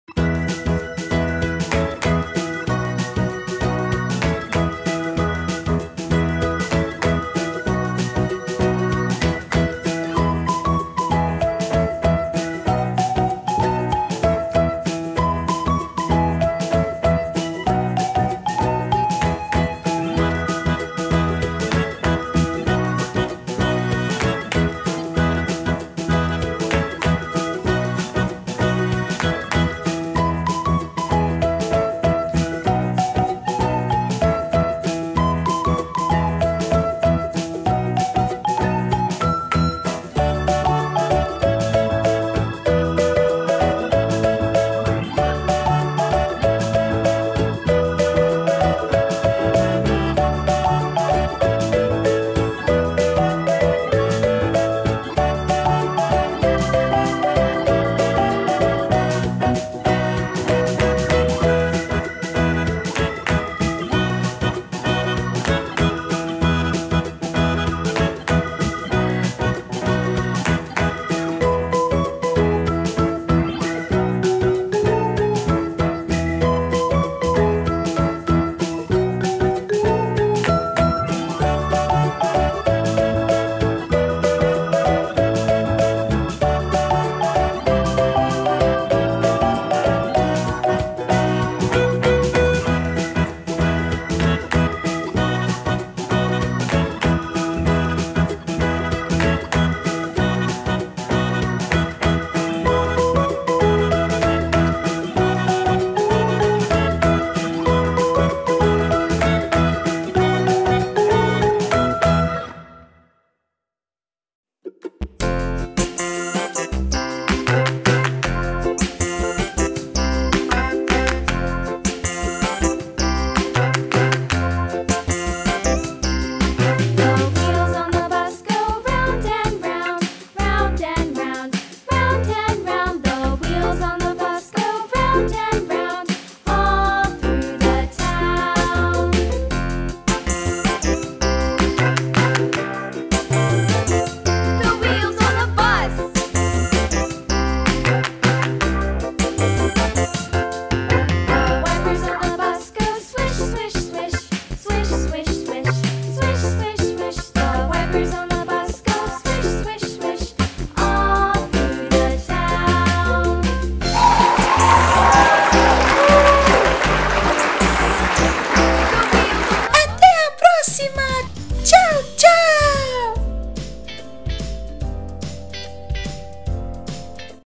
How to color Pocoyo and his friends/ Como Colorear pocoyó y sus amigos / Vamos Colorir o Pocoyo
Hoje vamos Colorir o Pocoyo com seus amigos
Today we are going to Color Pocoyo with your friends
आप अपने दोस्तों के साथ कलर या पोकोयो कैसे कर सकते हैं